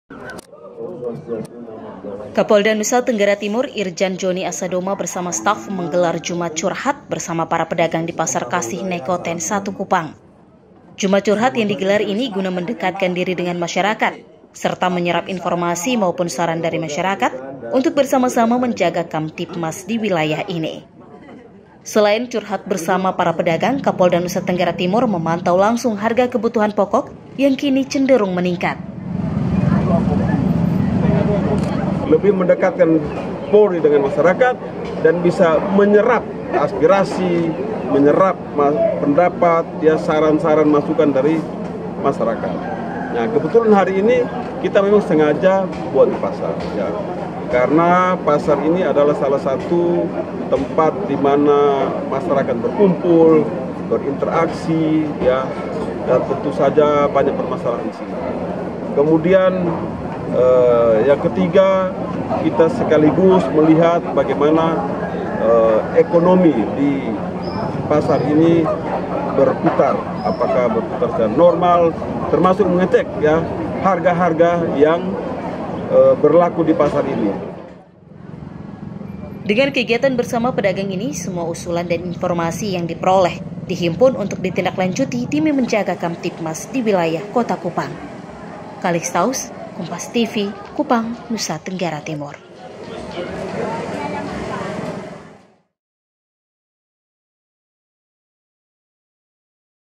Jumat Curhat, Kapolda NTT Dengar Langsung Keluhan Para Pedagang di Pasar Naikoten I
KUPANG, KOMPAS.TV - Kapolda Nusa Tenggara Timur, Irjen Jhoni Asadoma bersama staf menggelar Jumat Curhat bersama para pedagang di Pasar Kasih Naikoten I Kupang.

Jumat Curhat yang digelar ini guna mendekatkan diri dengan masyarakat serta  meyerap informasi maupun saran dari masyarakat untuk bersama-sama menjaga Kamtibmas di wilayah ini.

Selain curhat bersama para pedagang, Kapolda Nusa Tenggara Timur juga turut memantau langsung harga kebutuhan pokok yang kini cenderung meningkat.

Dengan kegiatan bersama pedagang ini, semua usalan dan informasi yang diperoleh akan dihimpun untuk ditindaklanjuti demi menjaga Kamtibmas di Wilayah Kota Kupang.